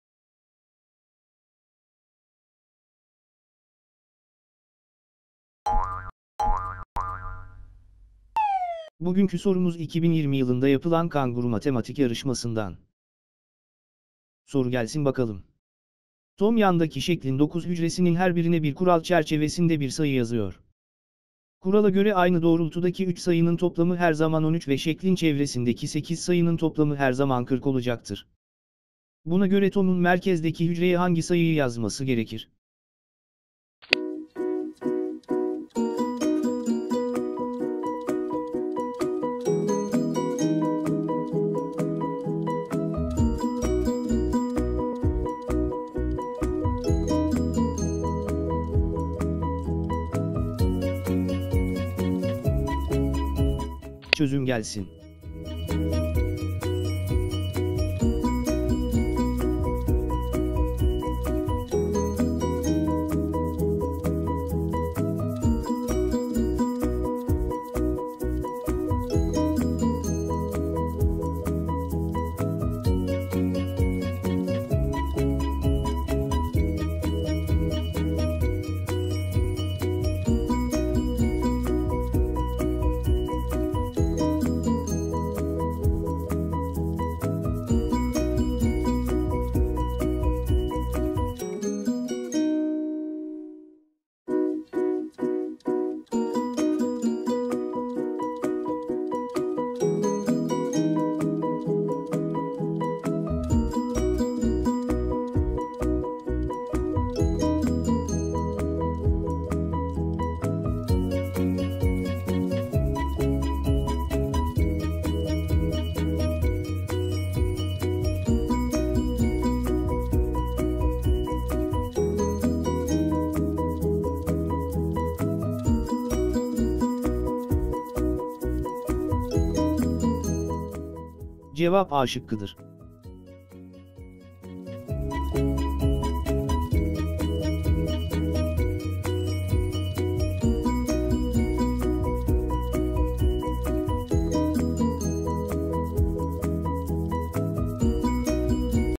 2020 Kanguru Matematik Soru ve Çözümleri -Junior- Sınıf 09/10
Tom, yandaki şeklin dokuz hücresinin her birine bir kural çerçevesinde bir sayı yazıyor. Kurala göre aynı doğrultudaki (çaptaki) üç sayının toplamı her zaman 13 ve şeklin çevresindeki sekiz sayının toplamı 40 olacaktır. Buna göre Tom'un merkezdeki hücreye hangi sayıyı yazması gerekir?